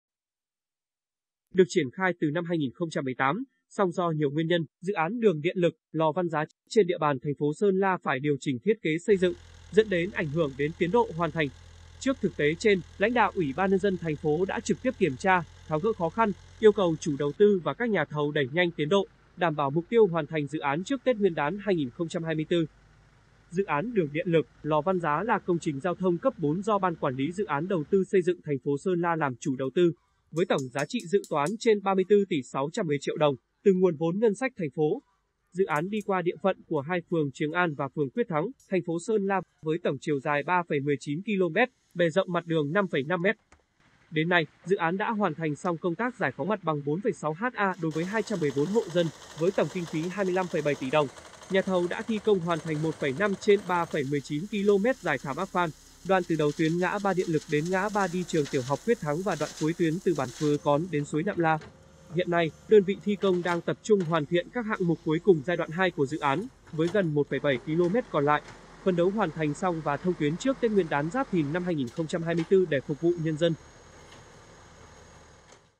Phấn đấu hoàn thành dự án đường Điện Lực - Lò Văn Giá trước Tết Nguyên Đán 2024
Được triển khai từ năm 2018, song do nhiều nguyên nhân, dự án đường Điện Lực - Lò Văn Giá trên địa bàn thành phố Sơn La phải điều chỉnh thiết kế xây dựng, dẫn đến ảnh hưởng đến tiến độ hoàn thành. Trước thực tế trên, lãnh đạo UBND thành phố đã trực tiếp kiểm tra, tháo gỡ khó khăn, yêu cầu chủ đầu tư và các nhà thầu đẩy nhanh tiến độ, đảm bảo mục tiêu hoàn thành dự án trước Tết Nguyên Đán 2024.